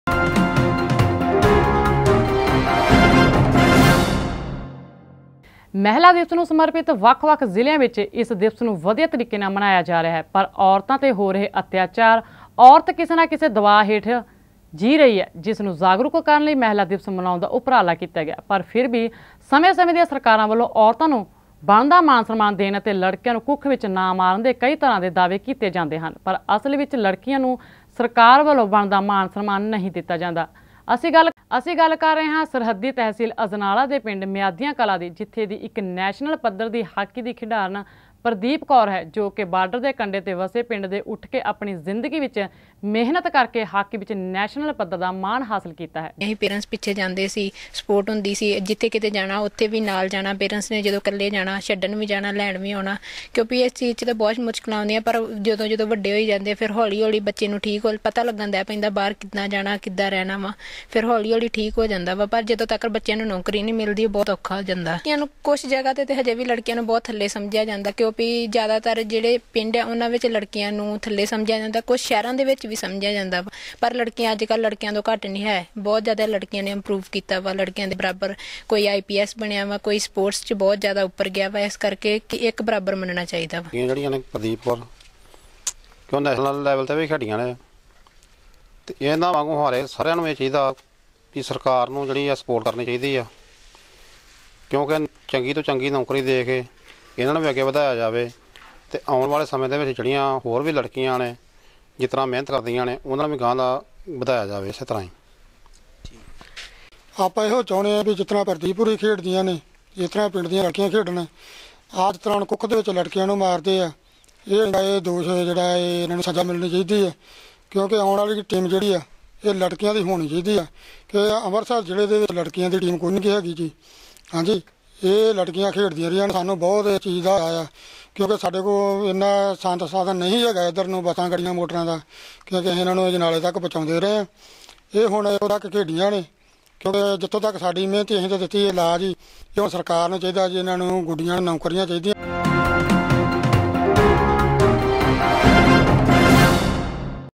Aone News|ਮਹਿਲਾ ਦਿਵਸ nu ਵਧੀਆਂ ਤਾਰੀਕੇ ਨਾਲ ਮਨਾਇਆਂ ਜਾ ਰਿਹਾ ਹੈ।
ਮਹਿਲਾ ਦਿਵਸ ਨੂੰ ਸਮਰਪਿਤ ਵੱਖ ਵੱਖ ਜਿਲ੍ਹਿਆਂ ਵਿੱਚ ਇਸ ਦਿਵਸ ਨੂੰ ਵਧੀਆਂ ਤਾਰੀਕੇ ਨਾਲ ਮਨਾਇਆਂ ਜਾ ਰਿਹਾ ਹੈ।ਪਰ ਔਰਤਾਂ ’ਤੇ ਹੋ ਰਹੇ ਅੱਤਿਆਚਾਰ,ਔਰਤ ਕਿਸੇ ਨਾ ਕਿਸੇ ਦਬਾਅ ਹੇਠ ਜੀ ਰਹੀ ਹੈ, ਜਿਸ ਨੂੰ ਜਾਗਰੂਕ ਕਰਨ ਲਈ ਮਹਿਲਾ ਦਿਵਸ ਮਨਾਉਣ ਦਾ ਉਪਰਾਲਾ ਕੀਤਾ ਗਿਆ।ਪਰ ਫਿਰ ਵੀ ਸਮਂੇ ਸਮੇਂ ਦੀਆਂ ਸਰਕਾਰਾਂ ਵੱਲੋ ਔਰਤਾਂ ਨੂੰ ਬਣਦਾ ਮਾਨ ਸਨਮਾਨ ਦੇਣ ਅਤੇ ਲੜਕੀਆਂ ਕੁੱਖ ਵਿਚ ਨਾ ਮਾਰਨ ਦੇ ਕਈ ਤਰ੍ਹਾਂ ਦੇ ਦਾਅਵੇ ਕੀਤੇ ਜਾਂਦੇ ਹਨ ਪਰ ਅਸਲ ਵਿਚ ਲੜਕੀਆਂ ਨੂੰ ਸਰਕਾਰ ਵੱਲੋ ਬਣਦਾ ਮਾਨ ਸਨਮਾਨ ਨਹੀ ਦਿੱਤਾ ਜਾਂਦਾ।
ਅਸੀ ਗੱਲ ਕਰ ਰਹੇ ਹਾਂ ਸਰਹੱਦੀ ਤਹਿਸੀਲ ਅਜਨਾਲਾ ਦੇ ਪਿੰਡ ਮਿਆਦੀਆਂ ਕਲ੍ਹਾਂ ਦੀ ਜਿੱਥੋ ਦੀ ਇਕ ਨੈਸ਼ਨਲ ਪੱਧਰ ਦੀ ਹਾਕੀ ਦੀ ਖਿਡਾਰਣ ਪ੍ਰਦੀਪ ਕੌਰ ਹੈ ਜੋ ਕਿ ਬਾਰਡਰ ਦੇ ਕੰਡੇ ਤੇ ਵੱਸੇ ਪਿੰਡ ਤੋ ਉੱਠ ਕੇ ਆਪਣੀ ਜਿੰਦਗੀ ਵਿਚ ਮਿਹਨਤ ਕਰਕੇ ਹਾਕੀ ਵਿਚ ਨੈਸ਼ਨਲ ਪੱਧਰ ਦਾ ਮਾਣ ਹਾਸਲ ਕੀਤਾ ਹੈ, ਜੋਕਿ ਅੱਜ ਕੱਲ ਇਕ ਜਲੰਧਰ ਦੀ ਨਿੱਜੀ ਅਕੈਡਮੀ ਵਿਚ ਖਿਡਾਰੀਆਂ ਨੂੰ ਕੋਚਿੰਗ ਦੇ ਰਹੀ ਹੈ ਅਤੇ ਨਾਲ ਨਾਲ ਹੀ ਆਪਣੇ ਘਰ ਦੇ ਕੰਮ ਵੀ ਕਰਦੀ ਹੈ ਪਰ ਸਰਕਾਰ ਵੱਲੋ ਇਸ ਲੜਕੀ ਪ੍ਰਦੀਪ ਕੌਰ ਨੂੰ ਅਣਗੋਲਿਆ ਕੀਤਾ ਜਾ ਰਿਹਾ ਹੈ।ਇੱਕ ਗਰੀਬ ਪਰਿਵਾਰ ਦੀ ਲੜਕੀ ਜੋ ਕਿ ਨੈਸ਼ਨਲ ਤੱਕ ਖੇਡ ਚੁੱਕੀ ਹੈ।ਉਸ ਵੱਲ ਸਰਕਾਰ ਦਾ ਧਿਆਨ ਹੀ ਨਹੀ ਜਾ ਰਿਹਾ।ਇਹ ਗਰੀਬ ਪਰਿਵਾਰ ਦੀ ਲੜਕੀ ਅਪਣੇ ਜਿਤਾ ਹੋਏ ਮੈਡਲਾ ਨੂੰ ਦੇਖਕੇ ਹੀ ਅਪਣੇ ਸੁਪਨਿਆਂ ਨੂੰ ਦਬਾ ਕੇ ਰੱਖ ਰਹੀ ਹੈ।ਜਦ ਕਿ ਸਰਕਾਰ ਨੂੰ ਚਾਹੀਦਾ ਹੈ  ਕਿ ਅਜਿਹੀਆਂ ਹੋਣਹਾਰ ਲੜਕੀਆਂ ਵੱਲ ਵੱਧ ਵੱਧ ਤੋਂ ਧਿਆਨ ਦੇਣਾ ਚਾਹੀਦਾ ਹੈ ਤਾਂ ਕਿ ਇਹਨਾਂ ਗਰੀਬ ਪਰਿਵਾਰ ਦੀਆਂ ਲੜਕੀਆ ਦੇ ਸੁਪਨੇ ਵੀ ਪੁਰੇ ਹੋ ਸਕਣ ਤੇ ਸਾਡੇ ਪੰਜਾਬ ਦਾ ਨਾਂਮ ਰੋਸ਼ਨ ਕਰ ਸਕਣ।ਤਾਂ ਹੀ ਇਸ ਮਹਿਲਾ ਦਿਵਸ ਨੂੰ ਇੱਕ ਸਹੀ ਟੀਚਾ ਮੰਨਿਆ ਜਾਵੇਗਾ।
ਇਸ ਸਬੰਧੀ ਗੱਲਬਾਤ ਕਰਦੇ ਹੋਏ ਪ੍ਰਦੀਪ ਕੌਰ ਅਤੇ ਉਸ ਦੇ ਪਰਿਵਾਰਕ ਮੈਬਰਾਂ ਨੇ ਦੱਸਿਆ ਕਿ ਸਰਕਾਰ ਖਿਡਾਰੀਆਂ ਨੂੰ ਉੱਚਾ ਚੁੱਕਣ ਦੇ ਦਾਅਵੇ ਤਾਂ ਕਰਦੀ ਹੈ ਪਰ ਅਸਲ ਵਿਚ ਖਿਡਾਰੀਆਂ ਨੂੰ ਬਣਦਾ ਮਾਨ ਨਹੀ ਦਿੱਤਾ ਜਾਂਦਾ ਸੋ ਸਰਕਾਰਾਂ ਨੂੰ ਚਾਹੀਦਾ ਹੈ ਕਿ ਲੜਕੀਆਂ ਨੂੰ ਬਣਦਾ ਹੱਕ ਦਿੱਤਾ ਜਾਵੇ ਤਾਂ ਜੋ ਲੜਕੀਆਂ ਉਤਸ਼ਾਹਿਤ ਹੋਕੇ ਆਪਣੀ ਜਿੰਦਗੀ ਵਿਚ ਖੇਡਾਂ ਵਿਚ ਇਕ ਚੰਗਾ ਮੁਕਾਮ ਹਾਸਲ ਕਰ ਸਕਣ।

Download

channel

What'saap no.94784-84087

Download

channel